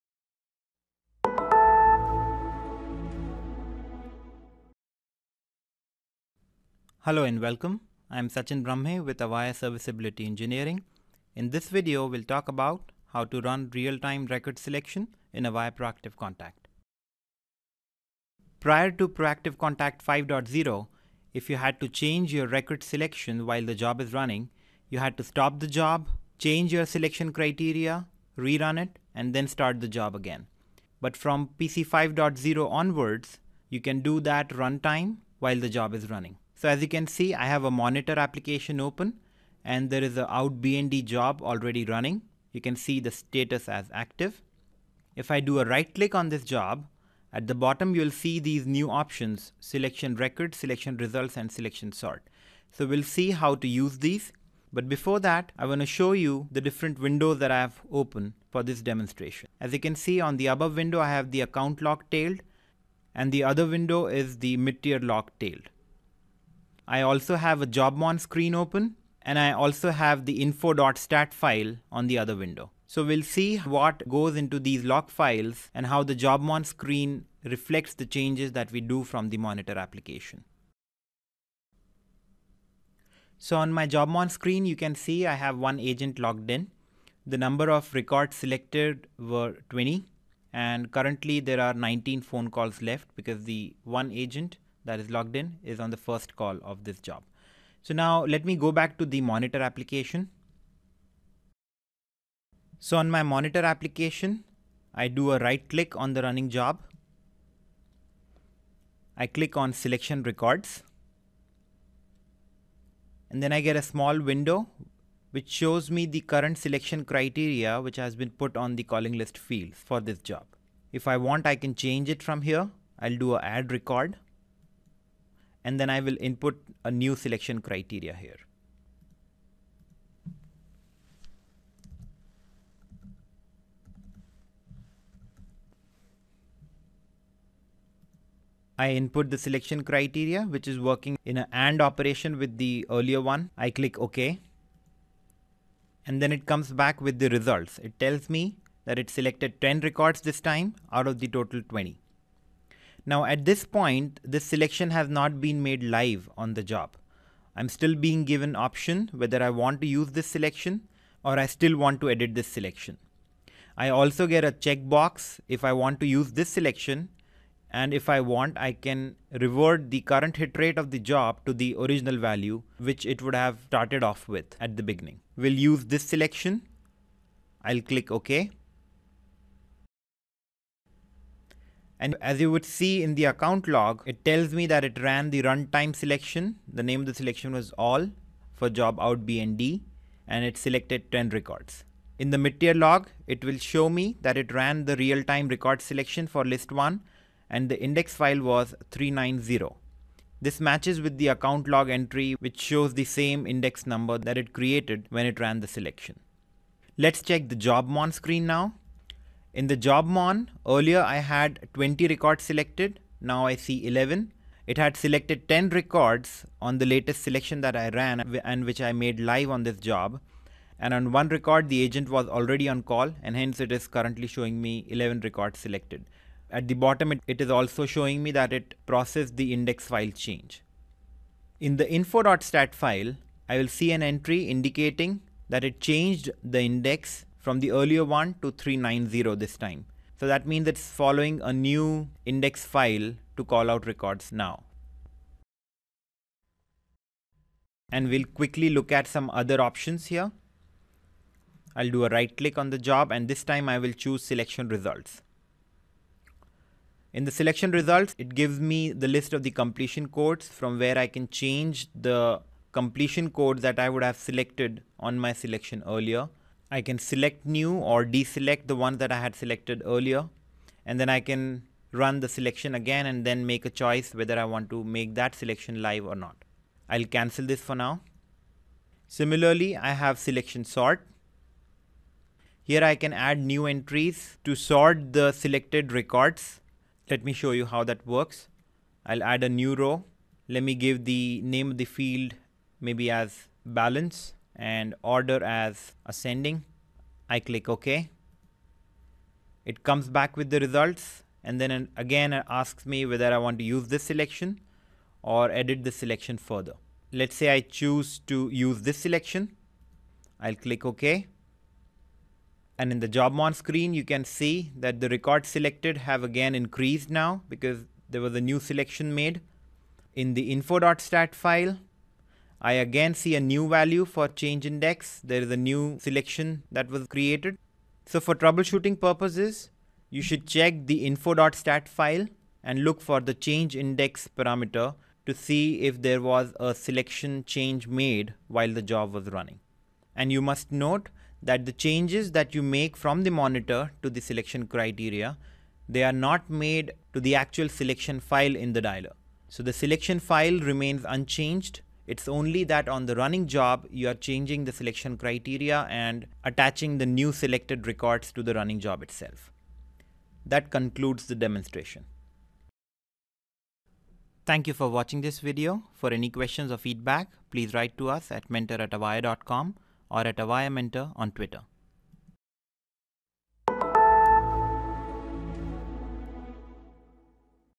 How to run Real-time Record Selection in Avaya Proactive Contact
This video demonstrates the new feature (Proactive Contact 5.0 onwards) where the record selection can be changed on the running job i.e. without having to stop the job, change the selection criteria, re-run it and then start the job again. Produced by Sachin Bramhe.